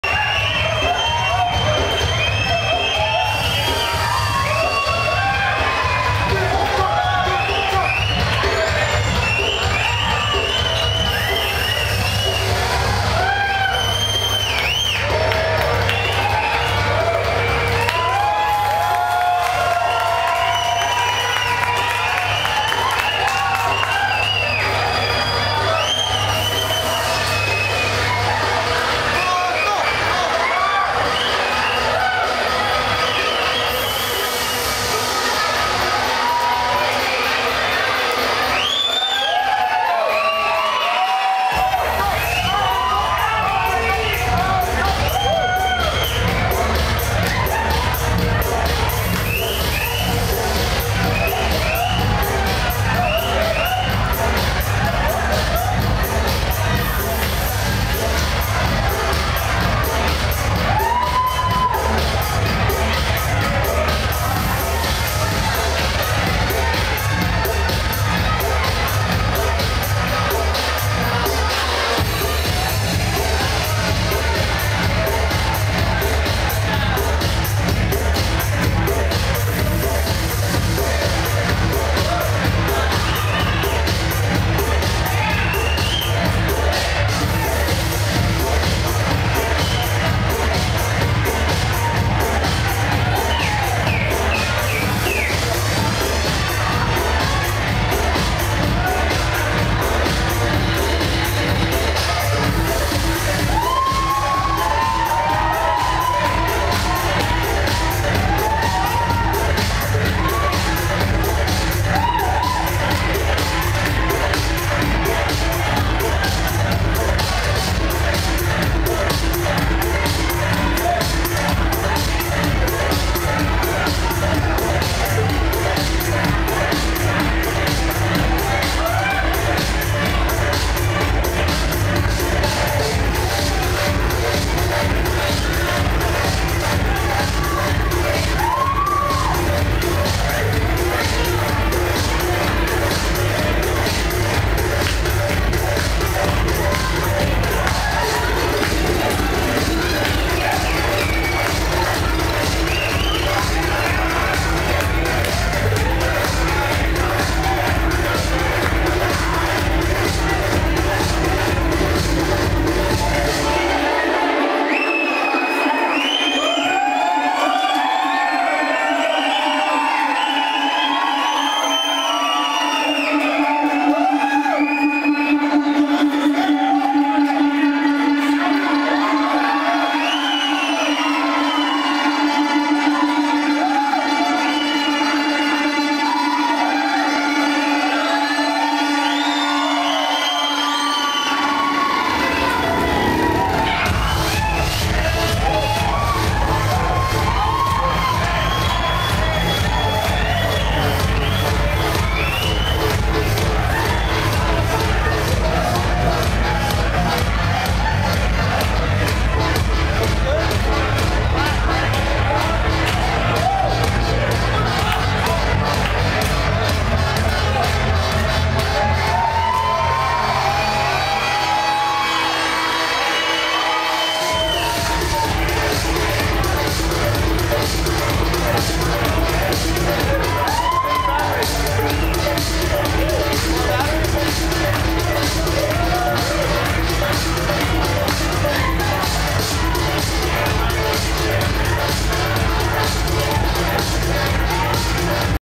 Siavash and Maxson @ Antinos, Lagos de Moreno
Siavash starting his set @ Lagos de Moreno, Jalisco Mx, Artica Party